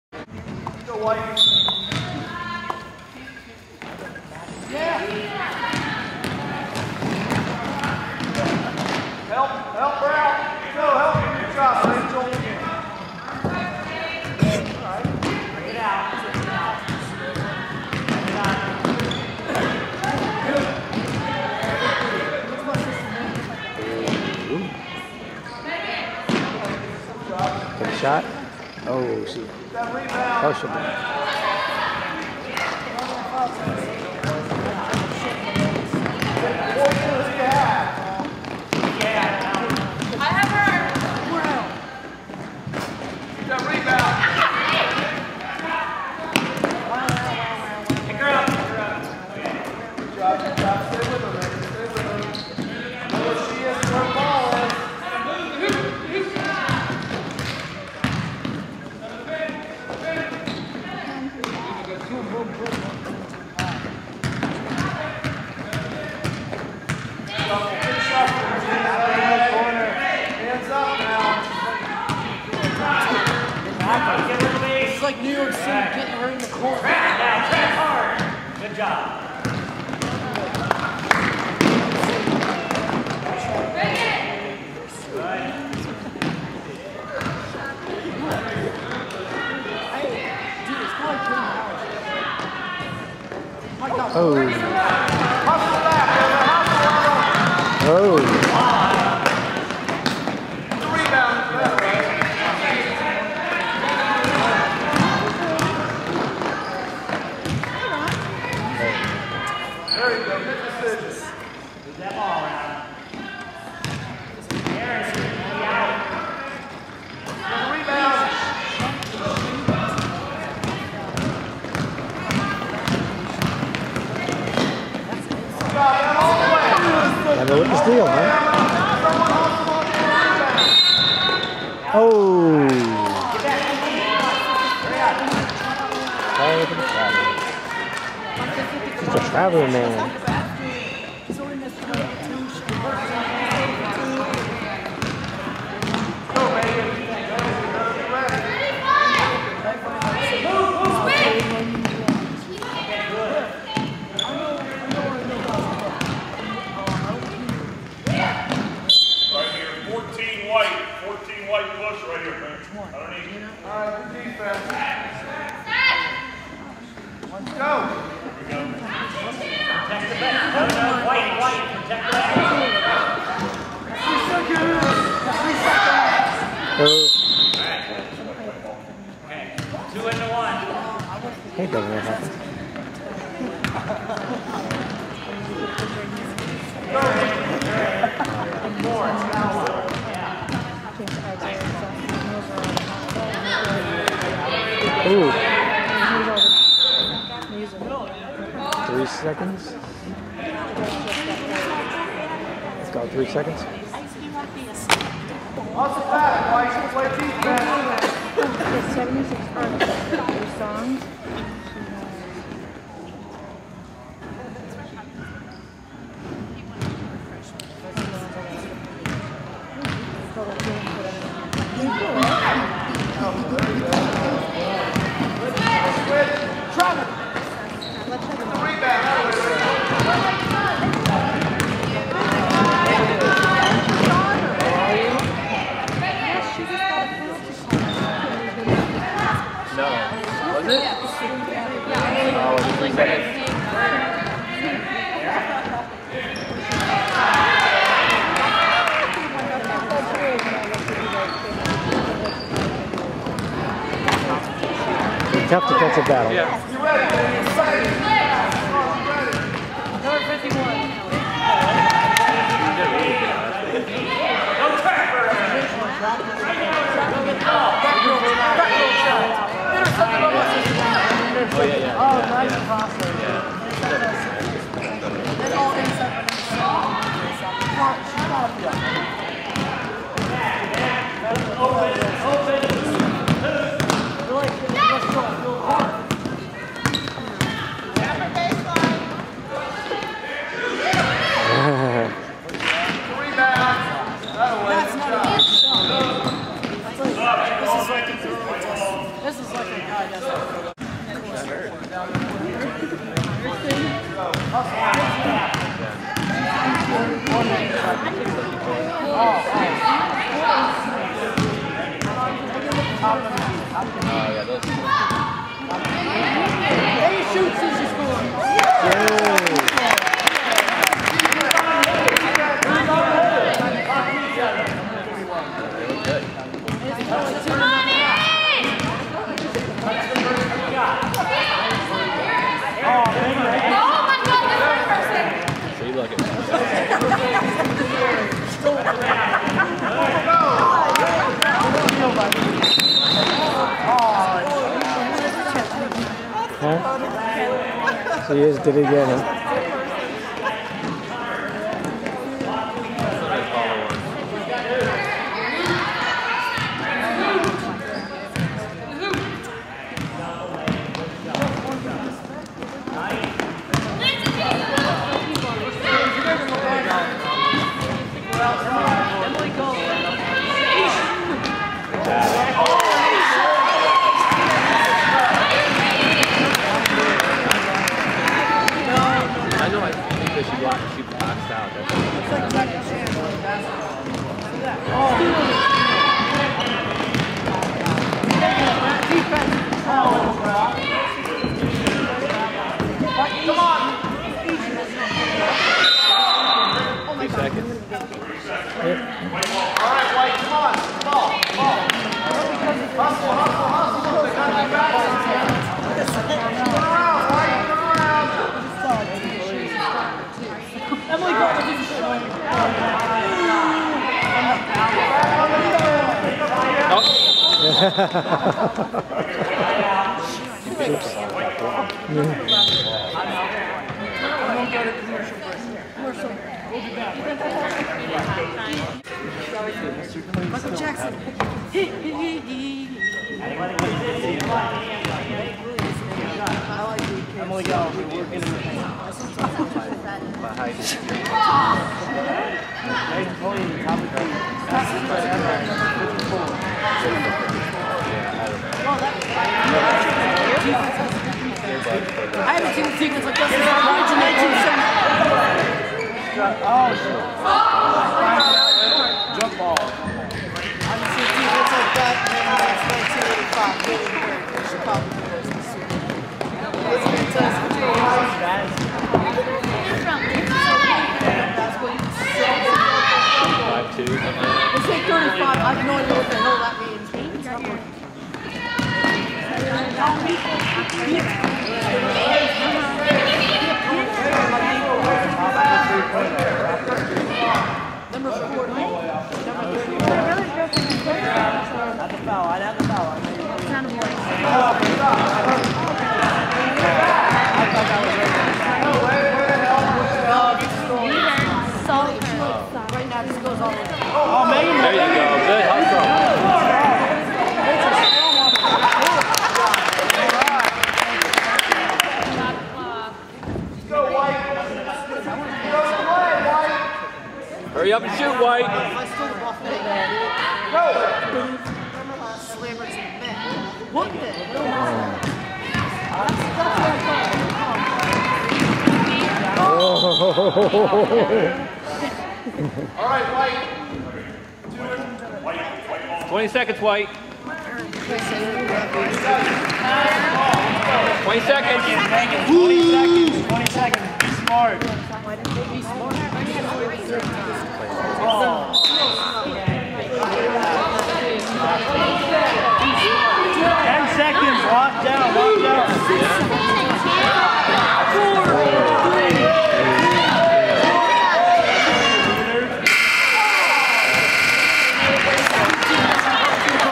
DYA Liberty 12-20-09 Q1
The Liberty play their third game of the year in the Dublin Youth Basketball League.   This is the first quarter played on Sun, 12/20/09 at Sells Middle School in Dublin, OH.